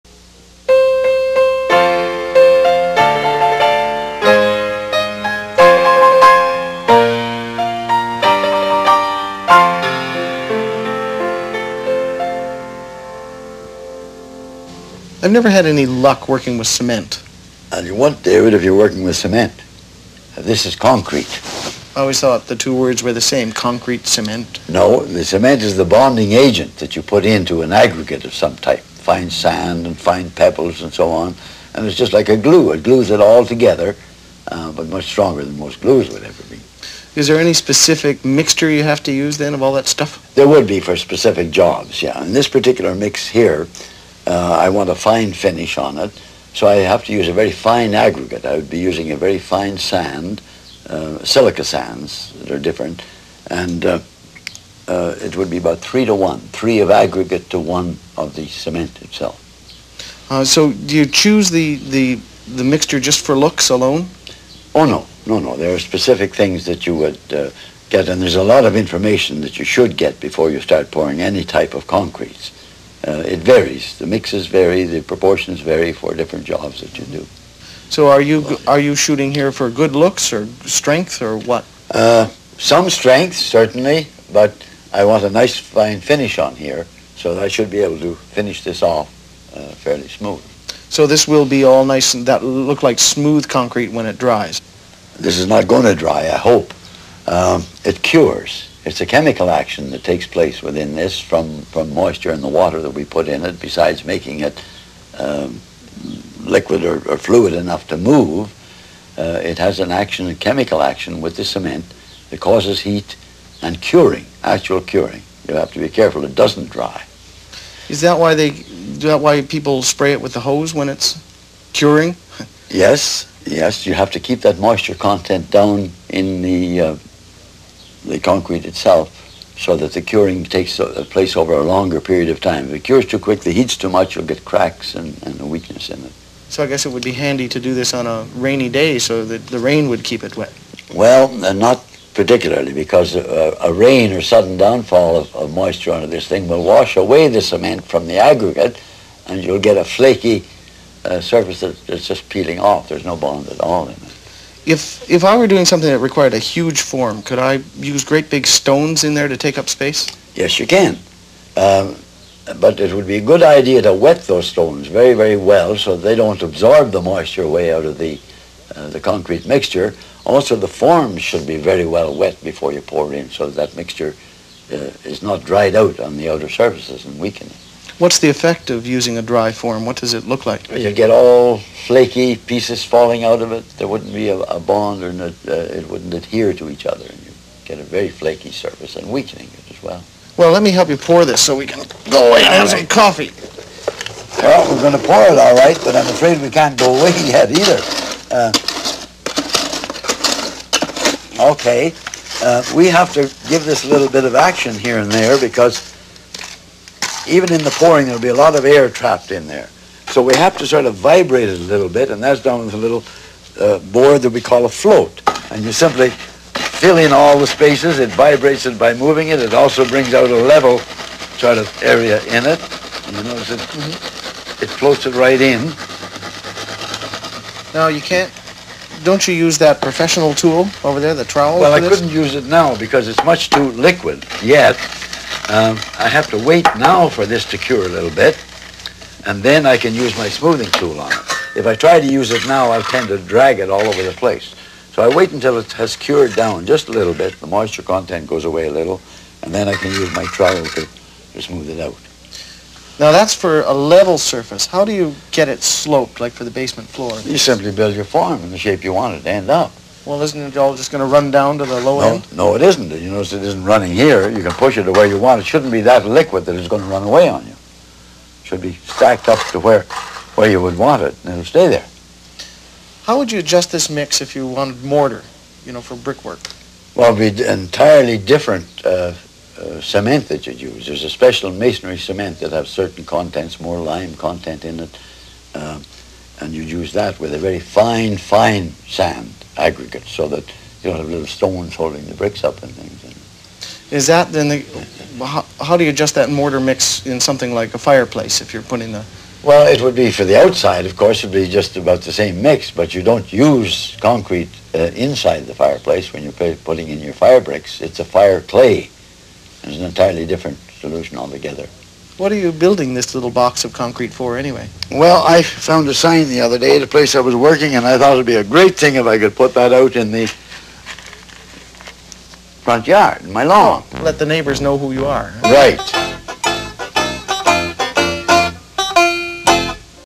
Concrete
Omer stringer explains working with concrete